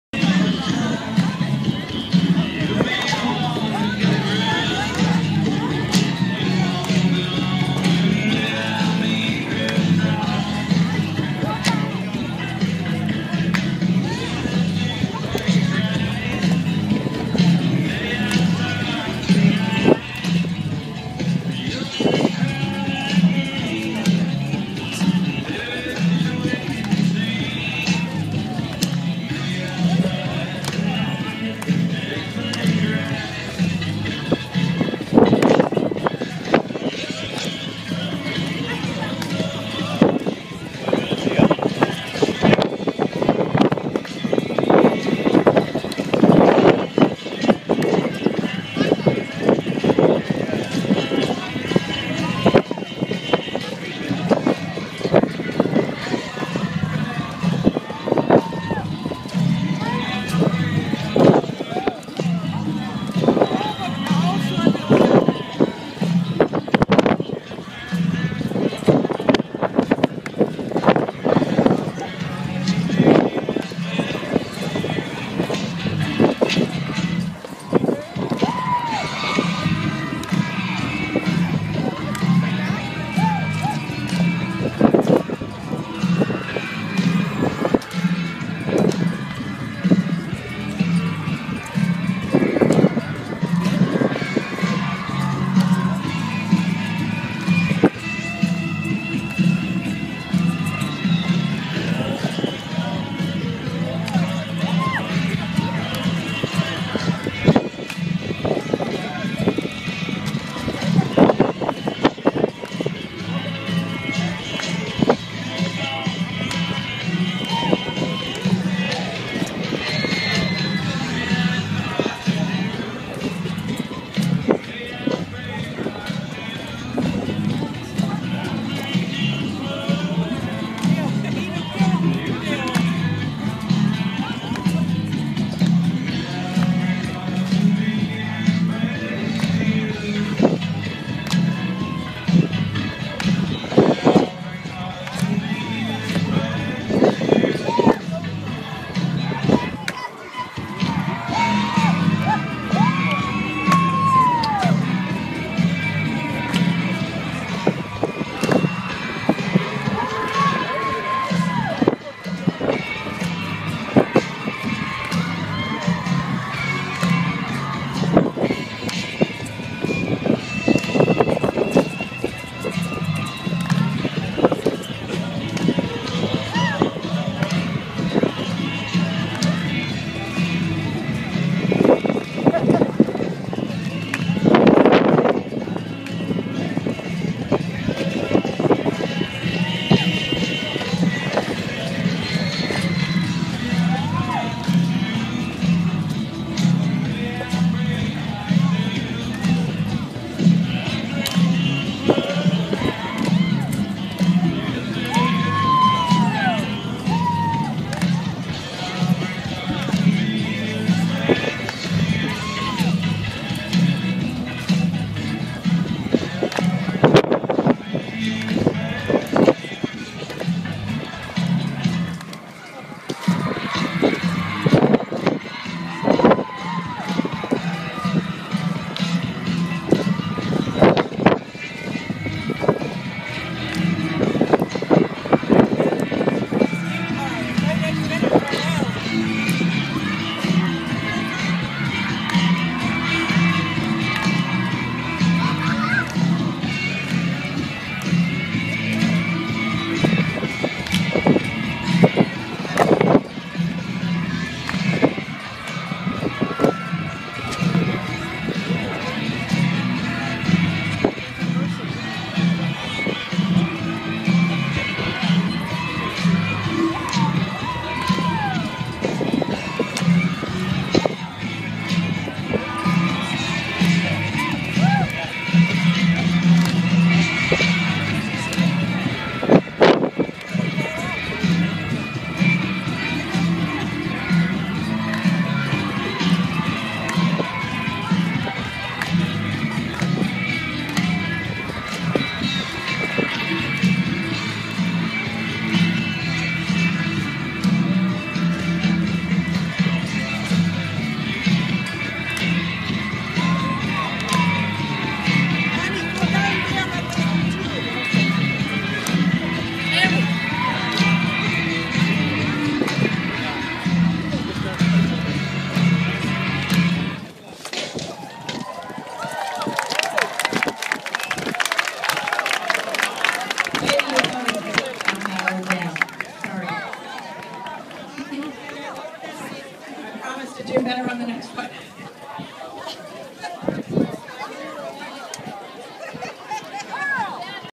Line Dancing on the London Bridge in Lake Havasu City, AZ. February 24th, 2013.